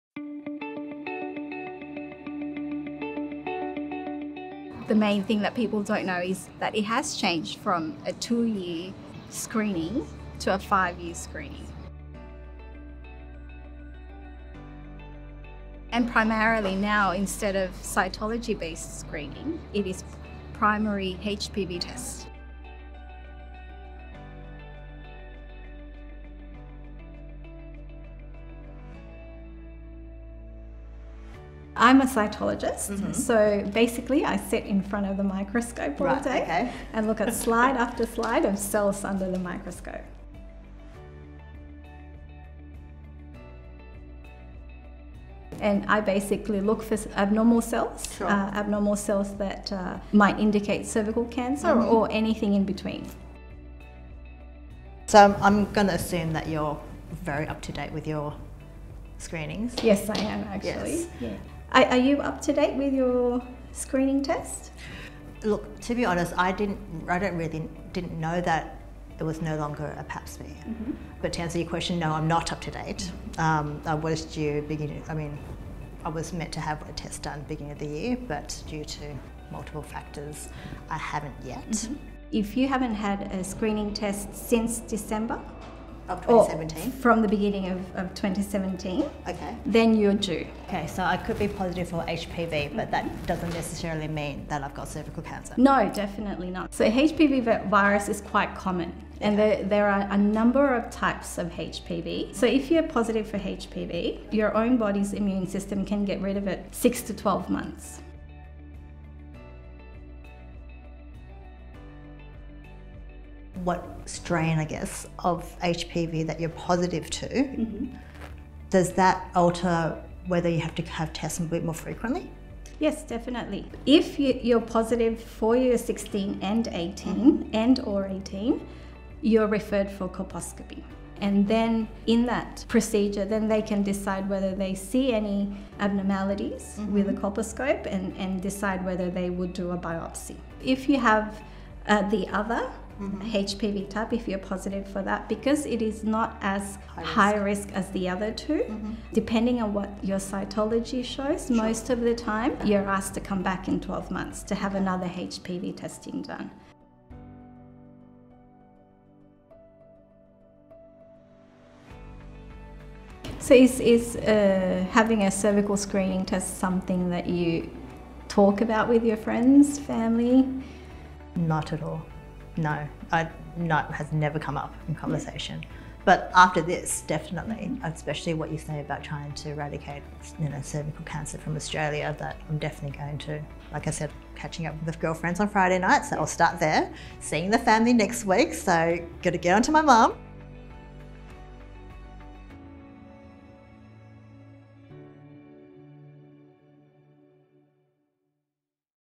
No more pap smears for Australian women; it's now a Cervical Screening Test - here's why
The pap smear test has changed to a Cervical Screening Test. Sydney resident Denise meets a cytologist to discuss why it's important to be up to date with cervical screening and some important facts about the test. Find out more about this and other pathology tests